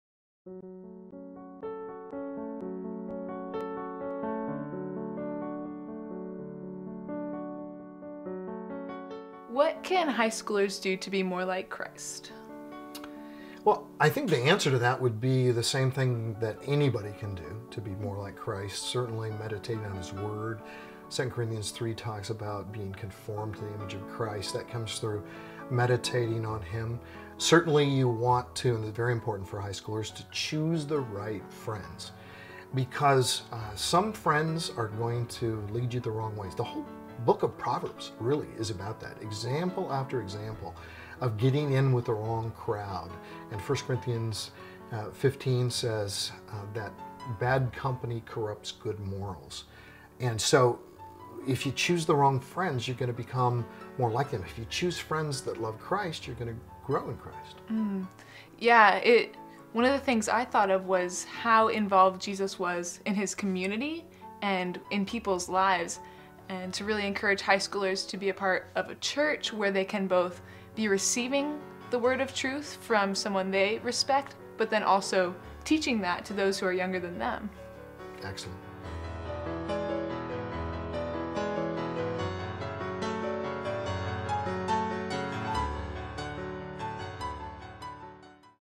What can a high school student do to be more like Christ?
Randy Alcorn answers this question in an interview with Julia Stager.